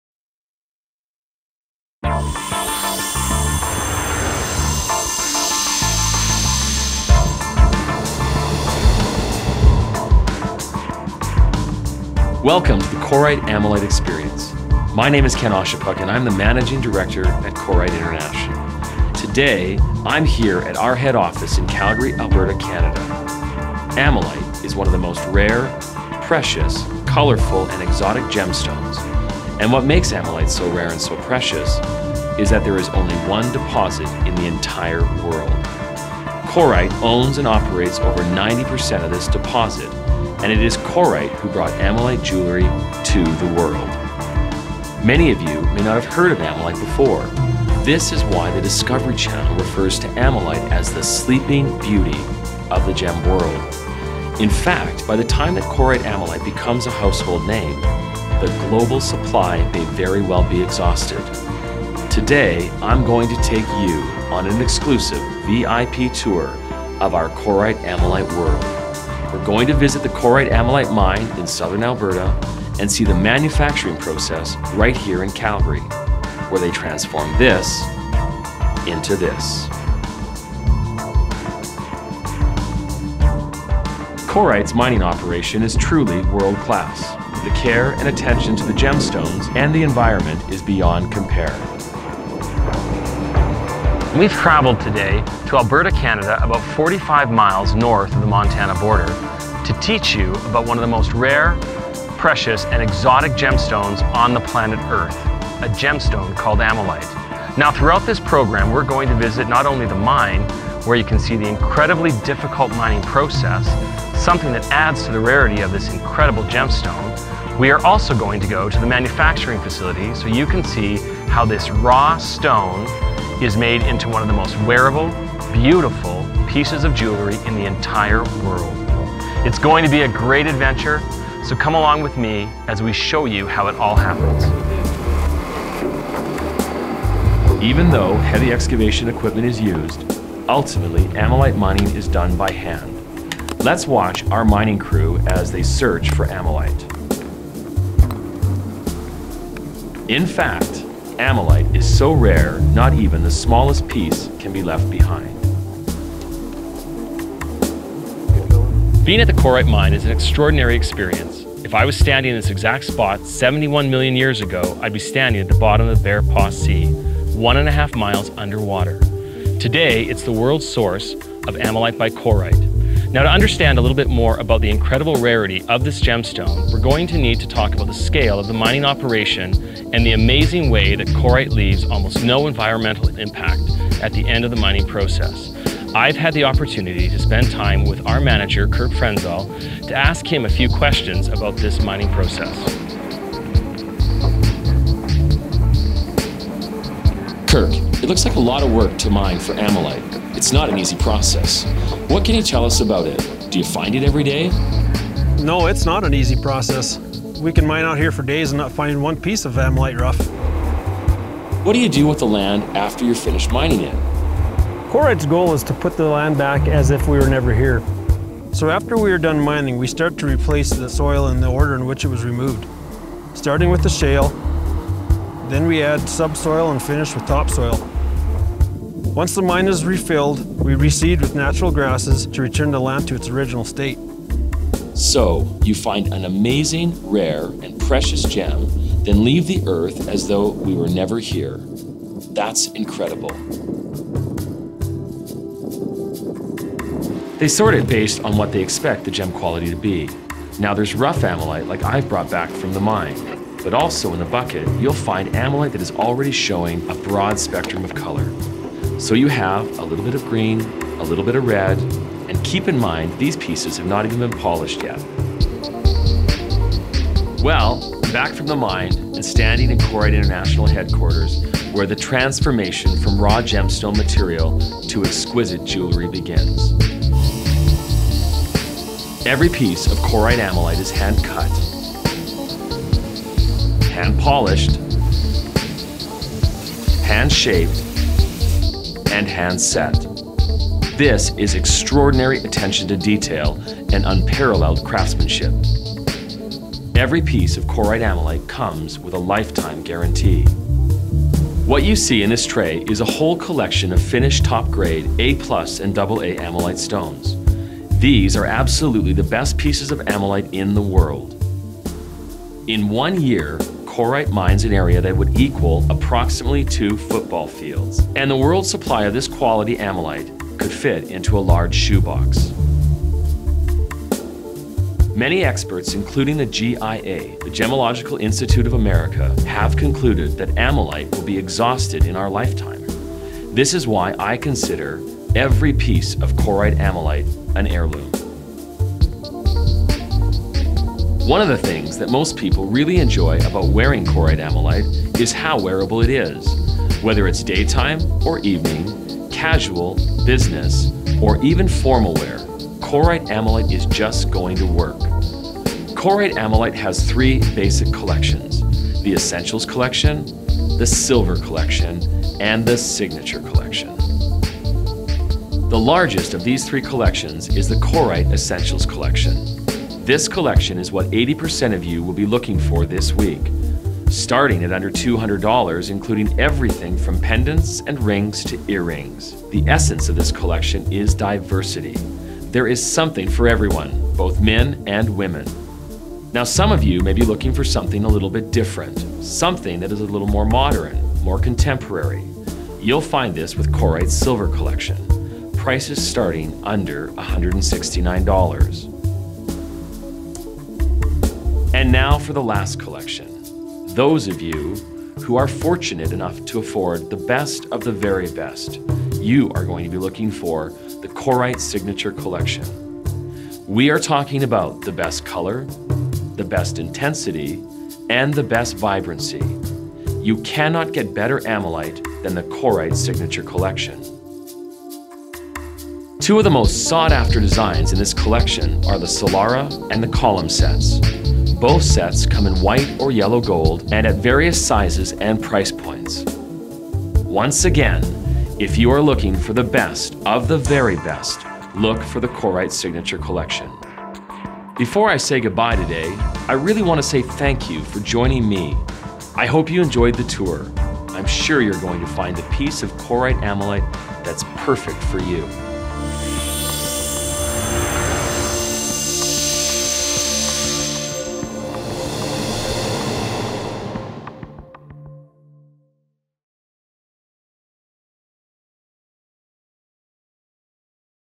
Ammolite Mine Tour
Ken Oschipok takes you on a tour showing you how Korite Ammolite is mined, processed and finally finished into stunning jewelry pieces that show off the vibrant and exciting qualities of this colourful and rare gemstone.